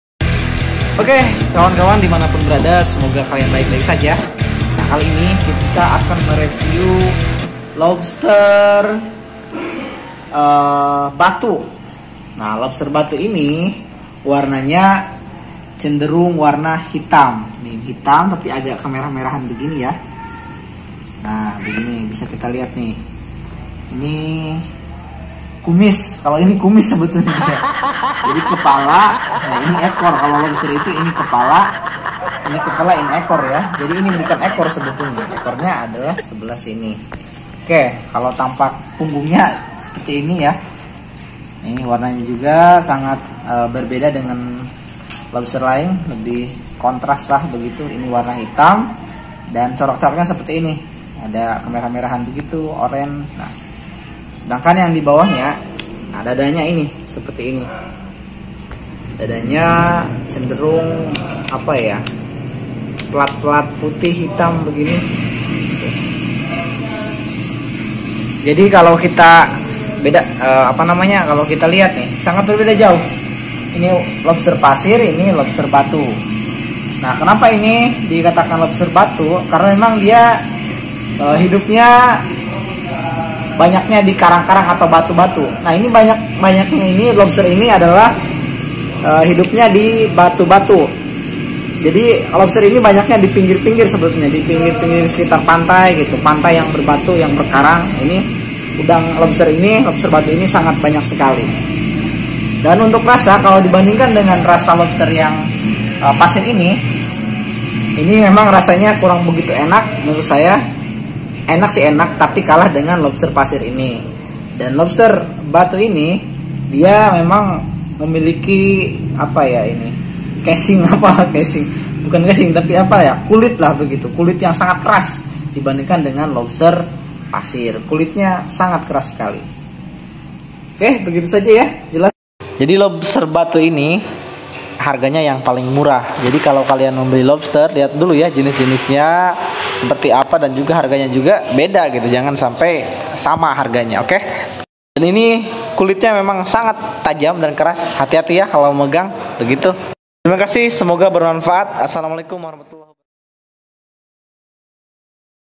💢ngereview lobster batu dan lobster pasir
ngereview ciri ciri udang lobster batu dan udang lobster pasir yang masing masing udang laut
perangkap, lobster, laut, alat tangkap, ambau, udang, karang, kepiting, rajungan, ikan, jaring, ramah lingkungan, lobster trap